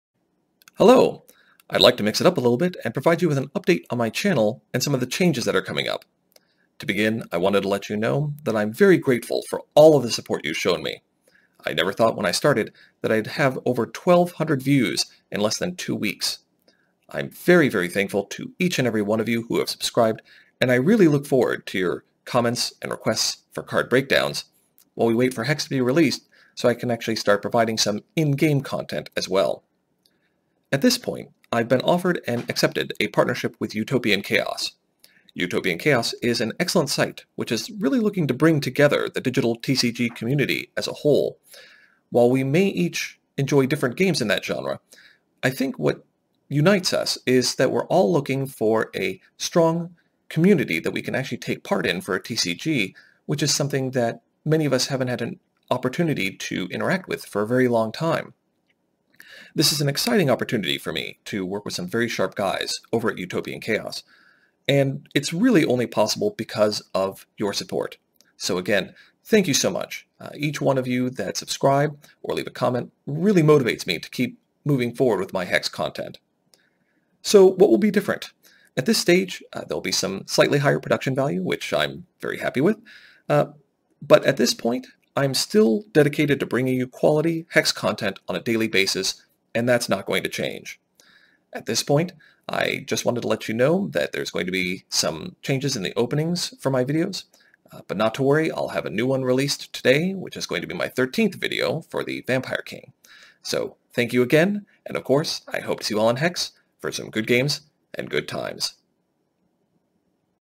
A quick update about changes coming to the channel!
Some recent developments have provided me with an opportunity to partner with the dTCG website Utopian Chaos!